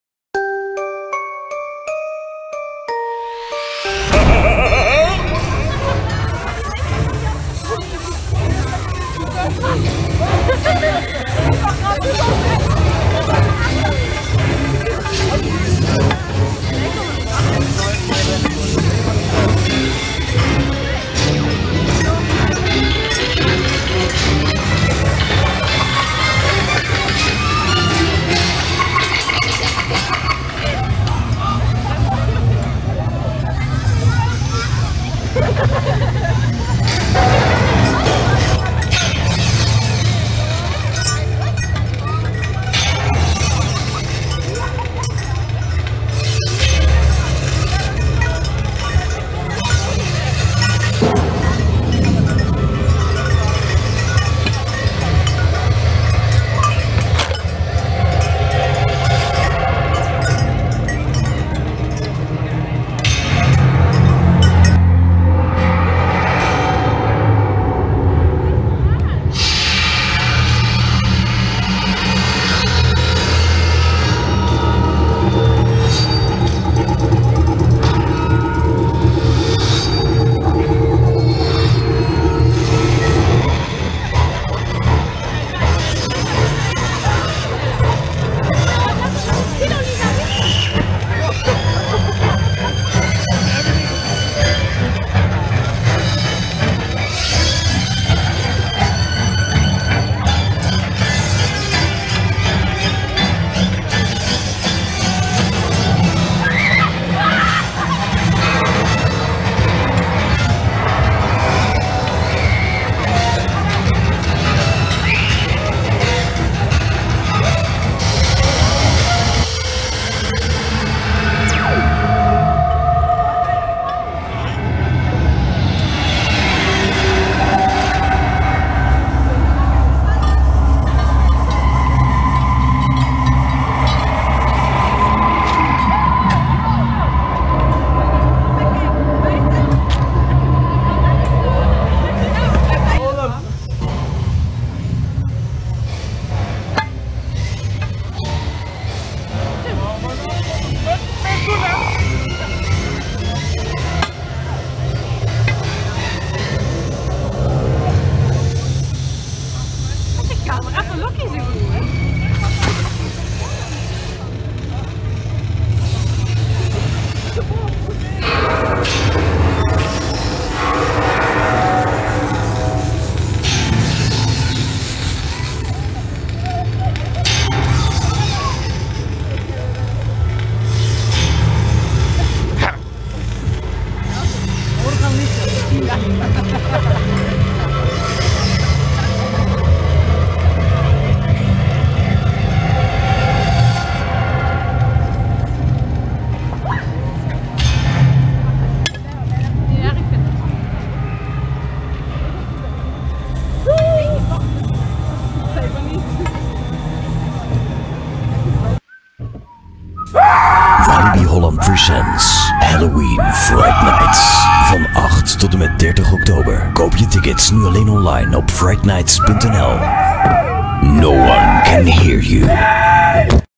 Halloween Fright Nights 2016 - Things (Vrijdag 14-10-2016)
HALLOWEEN FRIGHT NIGHTS 2016

Zombies in verre staat van ontbinding, kinky meesteressen, kapitein Blackbeard met zijn moordlustige bemanning en Eddie de Clown zorgen voor een overdosis ongekende horror tijdens het beste en meest gruwelijke evenement van Europa: Halloween Fright Nights in Walibi Holland. Je grootste nachtmerrie wordt werkelijkheid. Je schreeuwt om hulp, maar guess what: no one can hear you…

Van 8 t/m 30 oktober 2016, elke vrijdag, zaterdag en zondag, ben je nergens, maar dan ook nergens veilig in Walibi Holland. Vanaf 18:00 uur domineren de zombies, meesteressen, kapitein Blackbeard met zijn piraten en Eddie de Clown op het park. Bezoek tot 23:00 uur de Scare Zones, Haunted Houses en horrorshow.